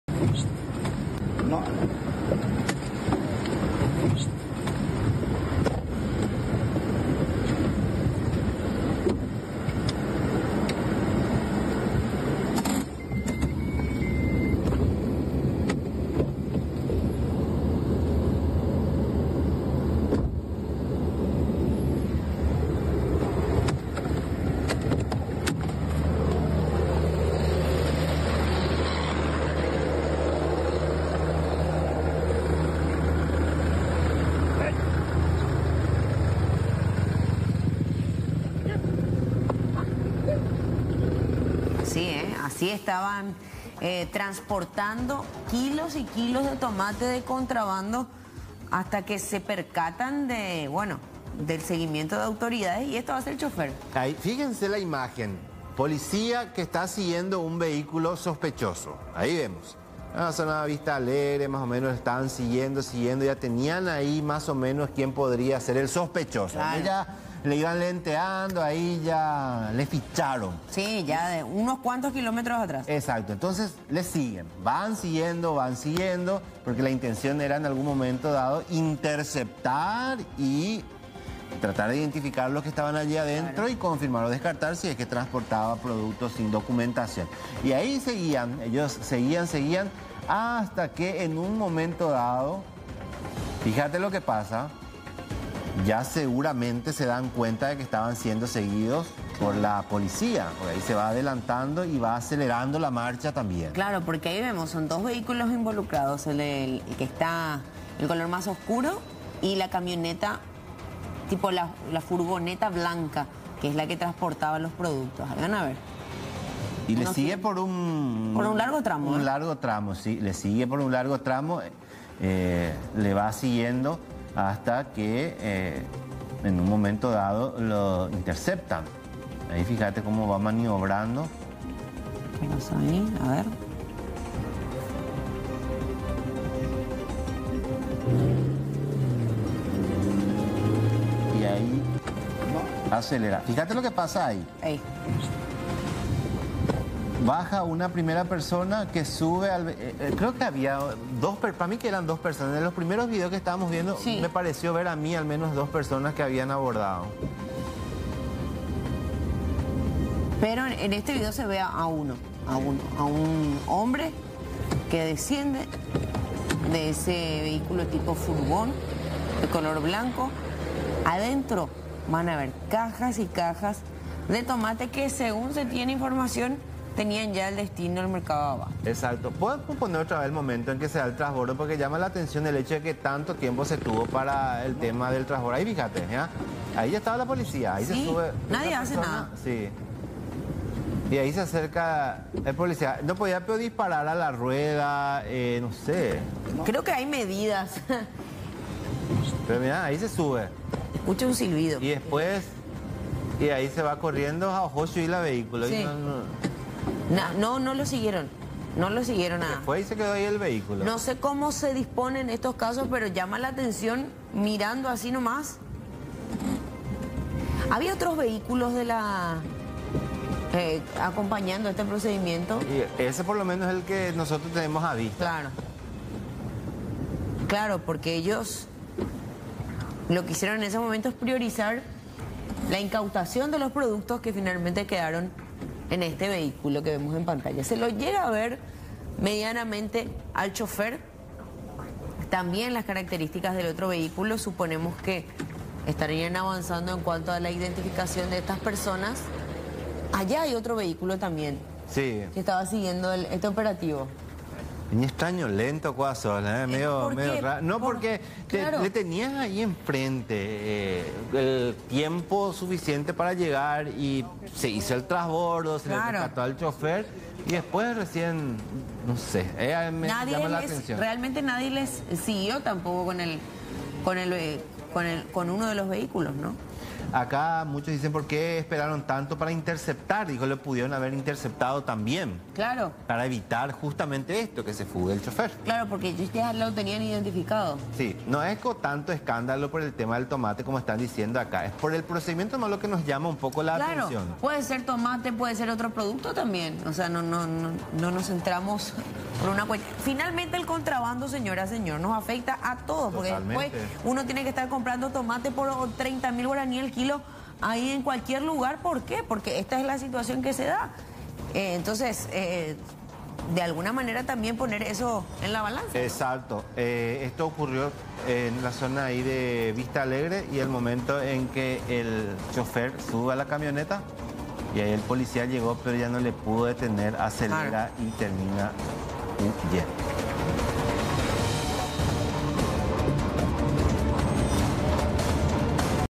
Tomate de contrabando
🍅🗣️ La policía estaba siguiendo a dos vehículos sospechosos hasta que al querer interceptarlos, el conductor del móvil blanco realiza un transbordo y finalmente escapa.